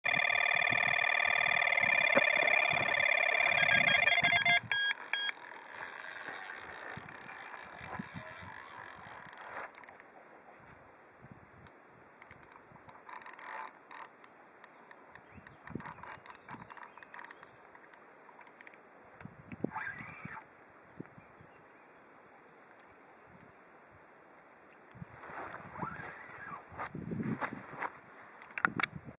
Départ carpe Delkim en direct
Petite vidéo départ en direct sur le lac de Besain